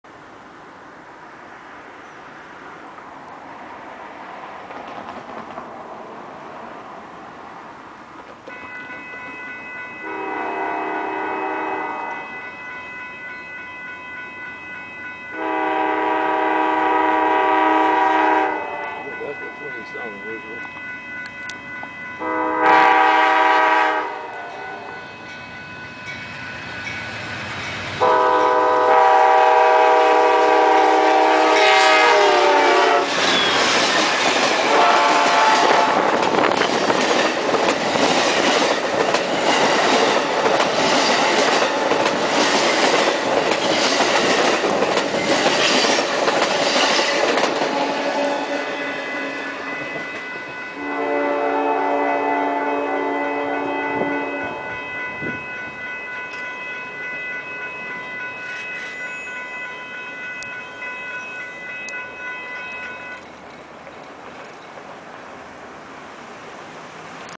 Eastbound California Zephyr leaving the trench at Sutro Street in Reno, Nevada, 12/23/2011
Eastbound California Zephyr leaving the trench at Sutro Street in Reno, 12/23/2011.